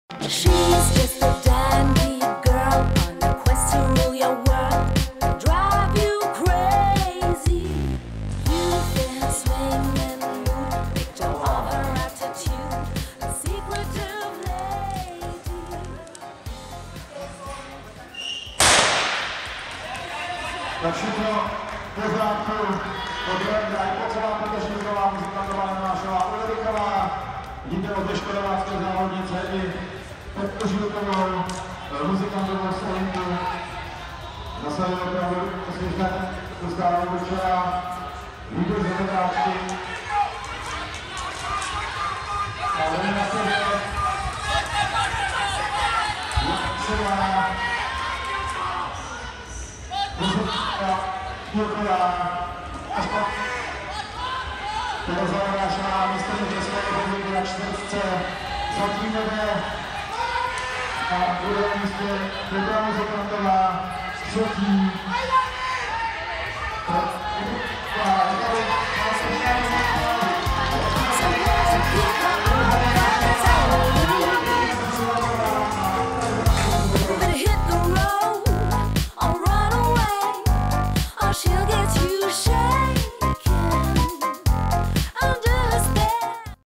2016_01_30_Přebor_Plzeň_400m_Ž_1.běh
1 Tereza Jonášová AK ŠKODA Plzeň 56.80
2 Petra Muzikantová PSK Olymp Praha 57.23
3 Tereza Petržilková AK ŠKODA Plzeň 57.81
4 Kateřina Ulrichová ASK Slavia Praha 58.75
5 Nikola Plocová AK Olymp Brno 65.59